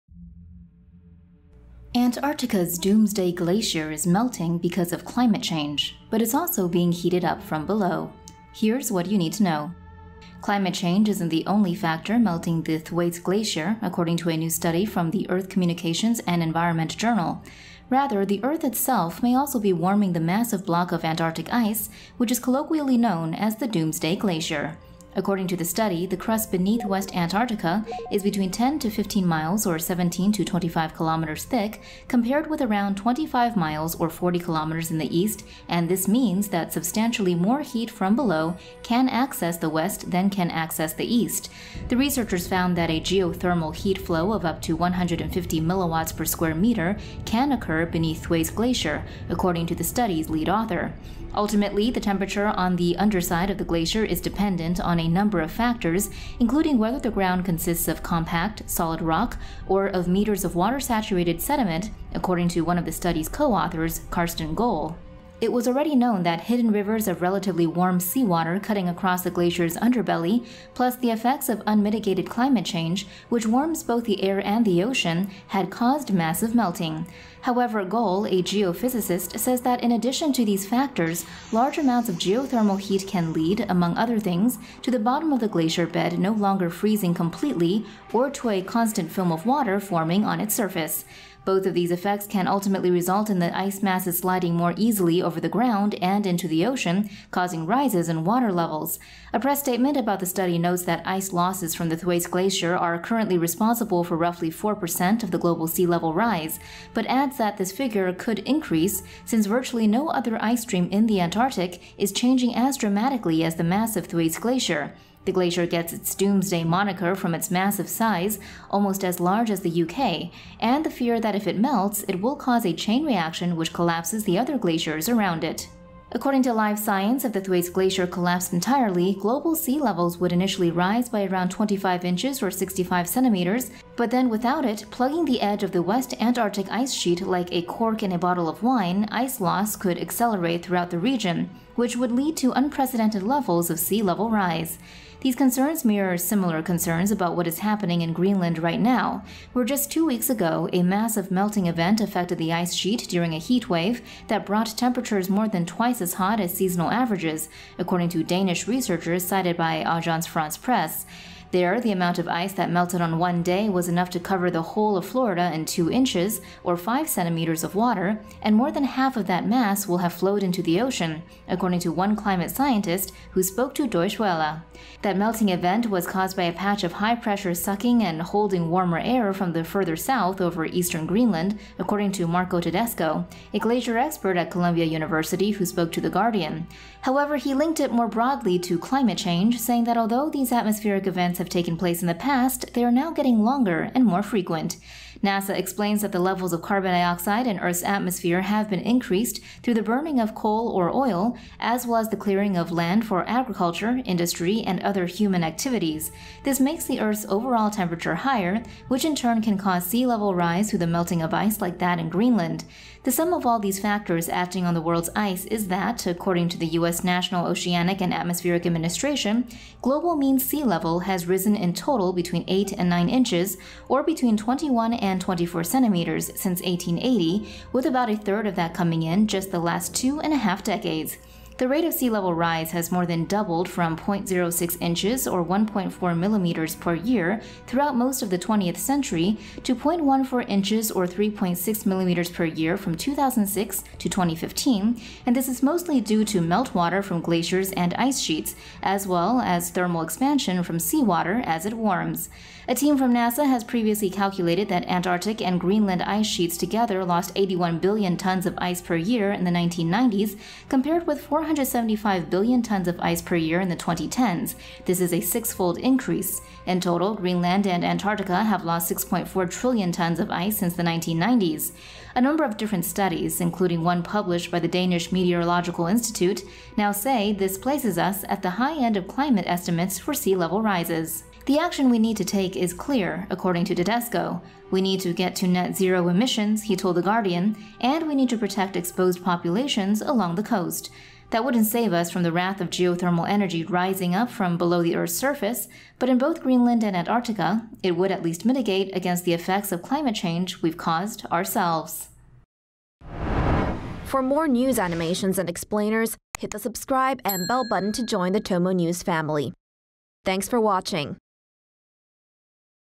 Earth Itself Warming 'Doomsday Glacier' on Top of Climate Change, Causing Massive Melting
Climate change isn’t the only factor melting the Thwaites Glacier, according to a new study from the Earth Communications and Environment journal. Rather, the Earth itself may also be warming the massive block of Antarctic ice, which is colloquially known as the "Doomsday Glacier."

According to the study, the crust beneath west Antarctica is between 10 to 15 miles or 17 to 25 kilometers thick compared with around 25 miles or 40 kilometers in the East and this means that substantially more heat from below can access the West than can access the East.

The researchers found that a geothermal heat flow of up to 150 milliwatts per square meter can occur beneath Thwaites Glacier, according to the study’s lead author.

SOURCES: NASA, Agence France Presse, Polar Portal, Nature, Deutsche Welle, NASA, The Guardian, Science Advances, Communications, Earth and Environment  journal, Alfred Wegener Institute, Science Magazine, Science Alert, Oceanographic Magazine, Live Science, Business Insider